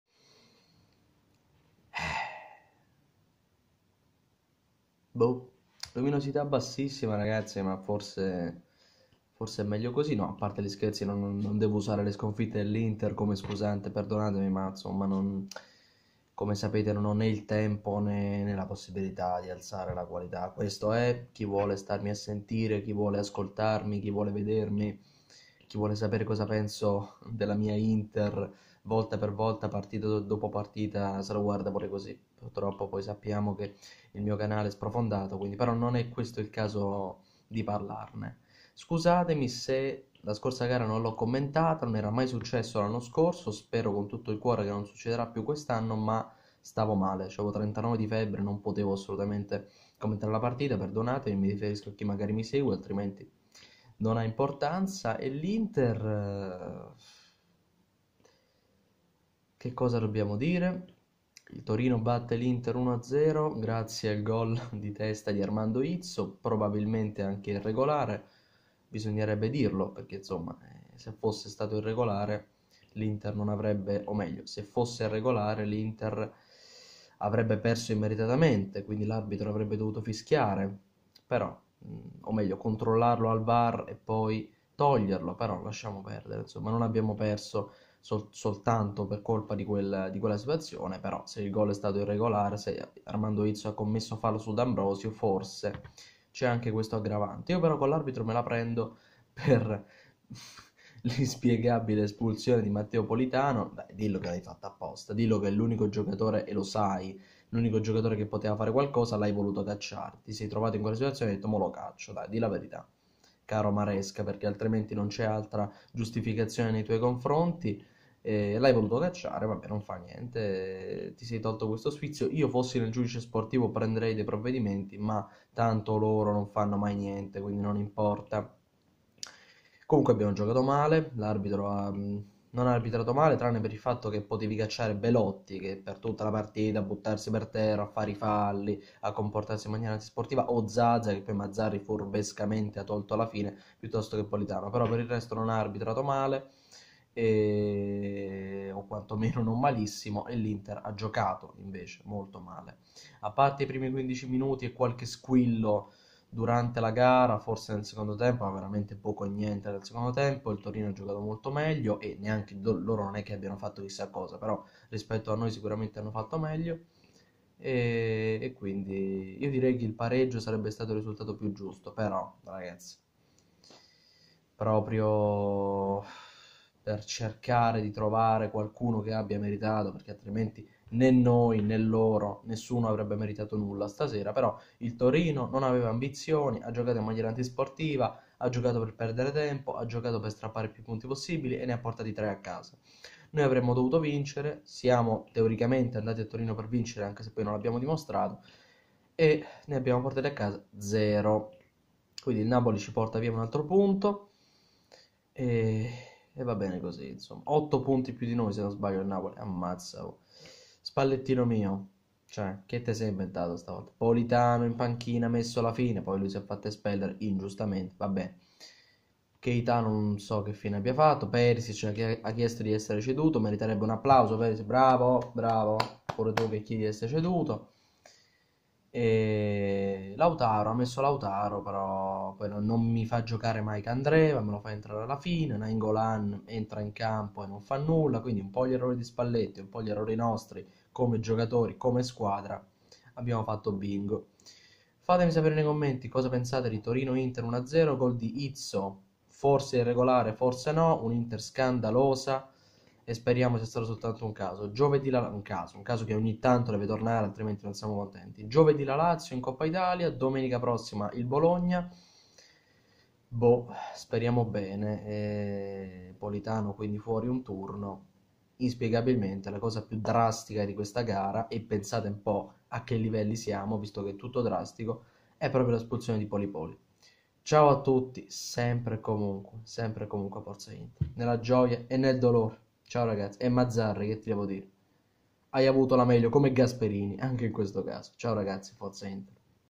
[SFOGO] Torino 1-0 INTER| SPALLETTI, COSÌ NON SI VA AVANTI! ANCHE L'ARBITRAGGIO PERÒ...
Buonasera a tutti ragazzi e benvenuti in questo nuovo video.
L'Inter cade a Torino e perde tre punti molto importanti, complice anche un arbitraggio rivedibile soprattutto nelle singole occasioni.
Cosa pensate di questa brutta sconfitta?

Spero che questo video vi sia piaciuto, se così è stato vi invito a lasciare un "mi piace", a commentare e ad iscrivervi al canale, se non lo avete ancora fatto. Sempre e comunque, forza Inter!